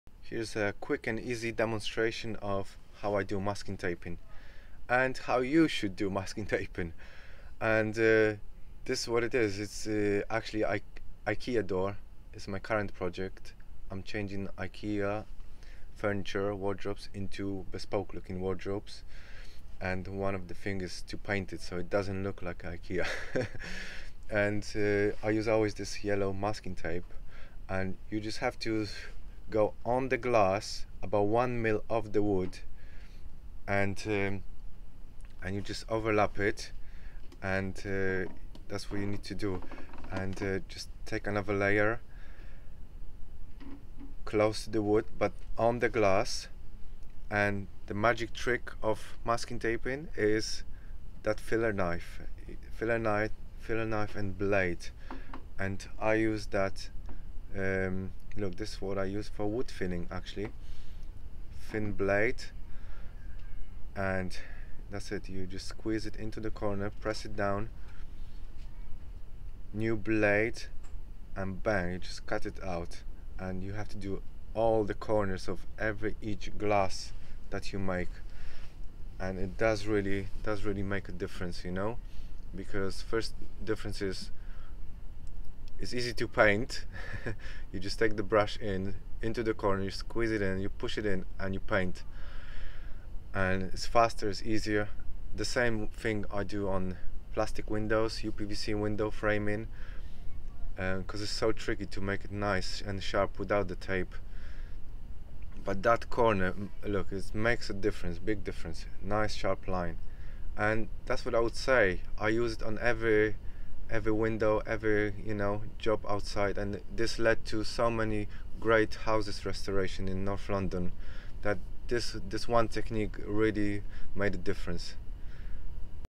How to: masking taping
Here is my technique of Masking taping, no one showed me this before so i kind of found it myself how to perfecly masking tape windows. I havent invented this method either but it is the best way.
And its not joke you will go up in ranks in your town after you paint windows perfecly :) Thats how i got paint many many beautifull houses in very small area in London. House after house only becasue the results of outside painting were outstanding.

Peace and love guys!